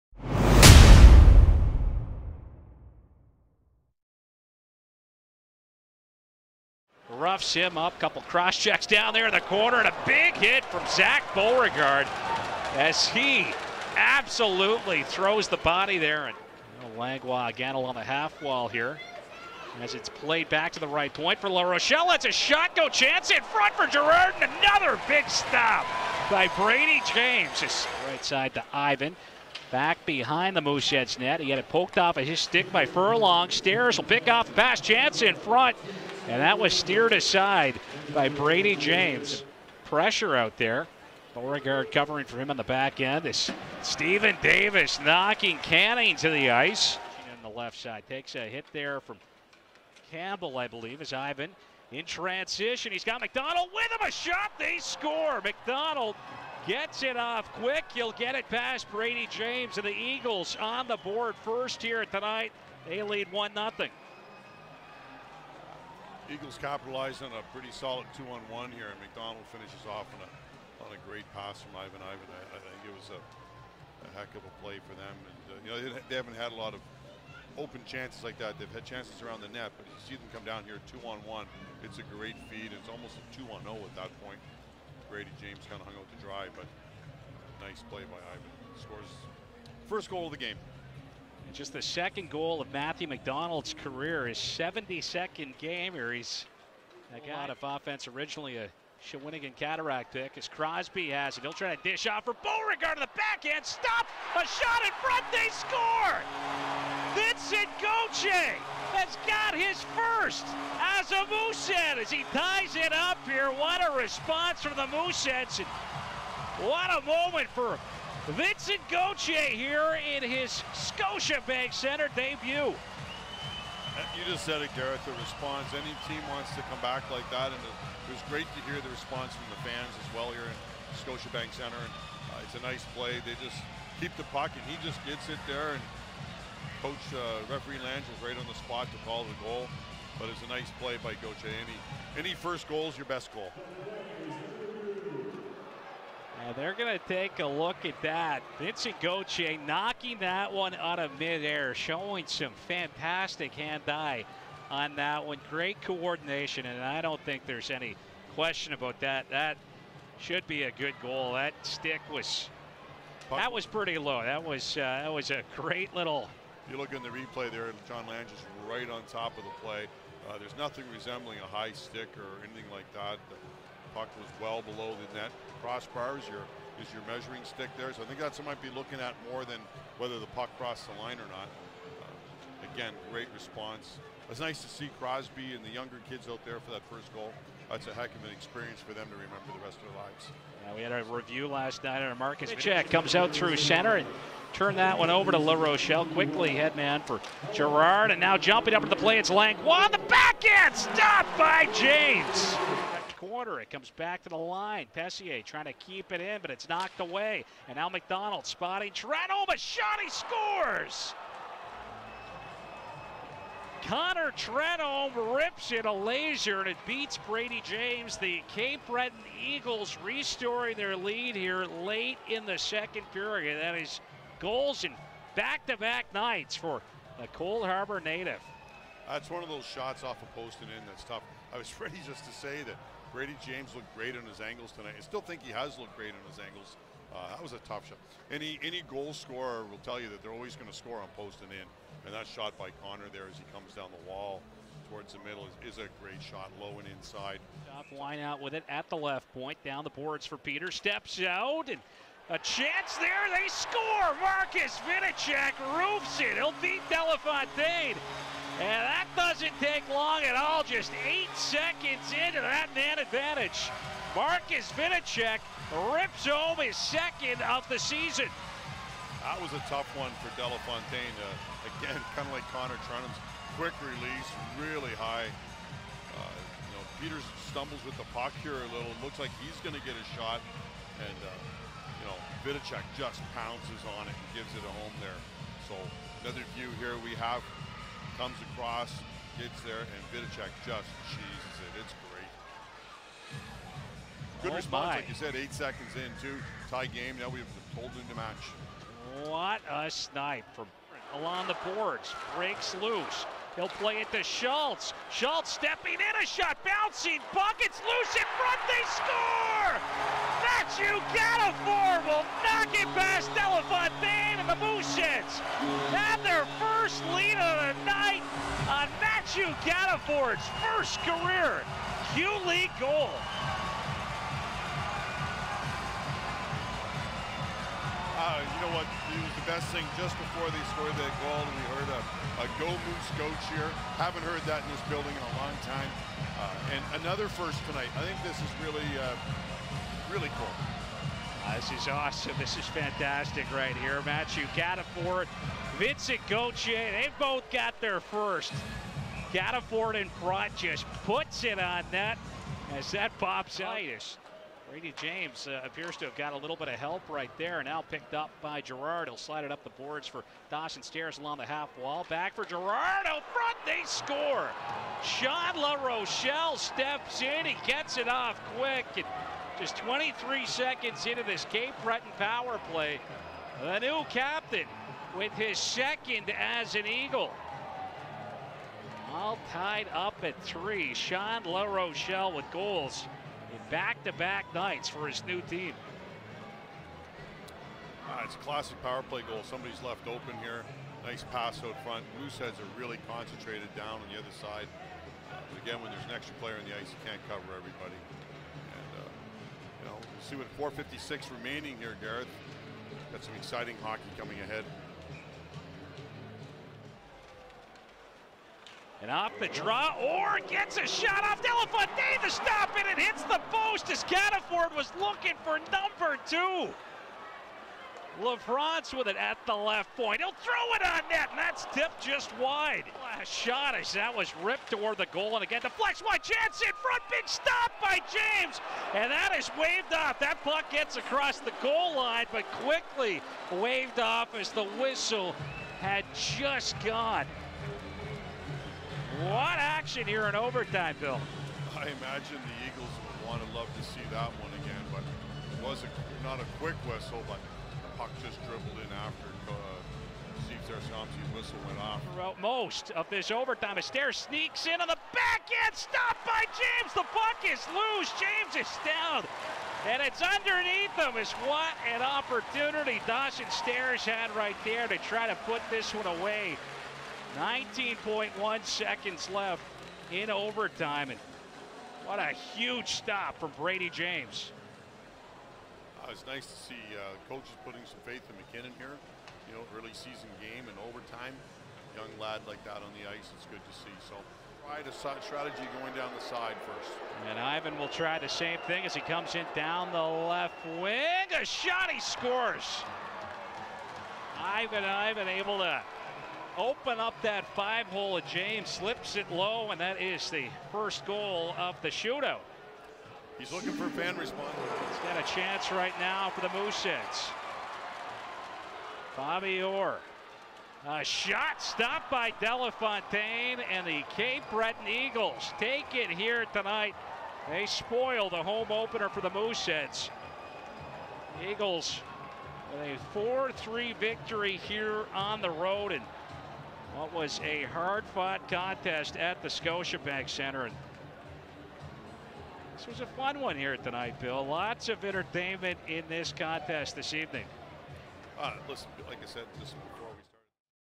Highlights: Mooseheads vs Eagles - Oct. 2nd, 2021
Cape Breton picks up a 4-3 shootout victory as Mathieu Cataford and Vincent Gauthier score their 1st career goals for Halifax.